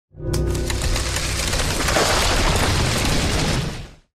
BassPro Crumble Intro v1
Introduction I created for BassPro241 using the designs of Andrew Kramer.